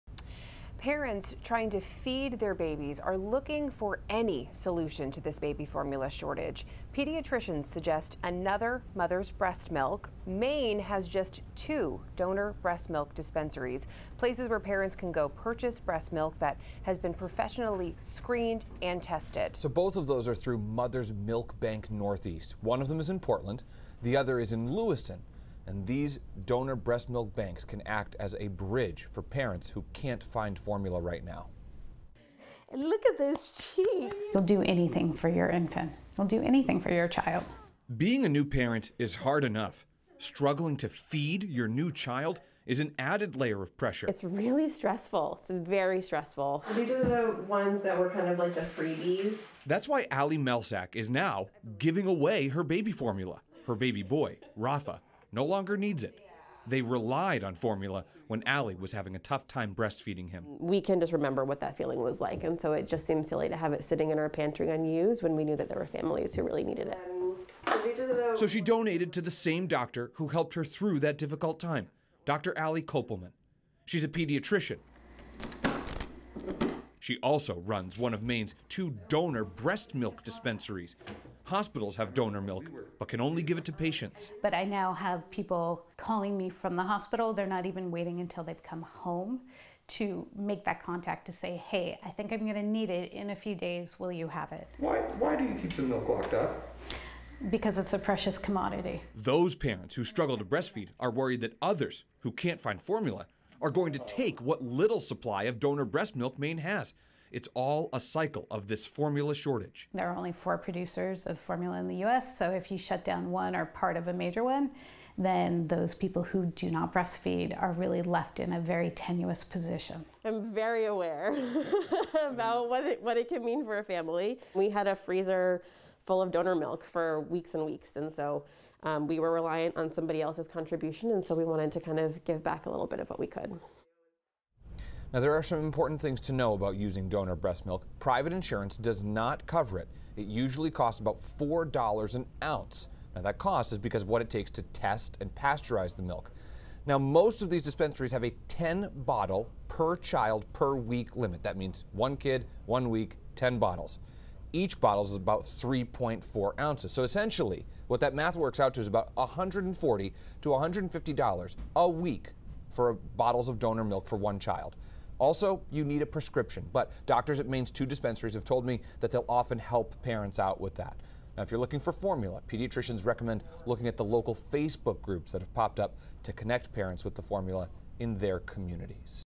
Maine's donor breast milk dispensaries help parents amid formula shortage
Maine has two donor breast milk dispensaries through Mother's Milk Bank Northeast, a nonprofit based in Massachusetts.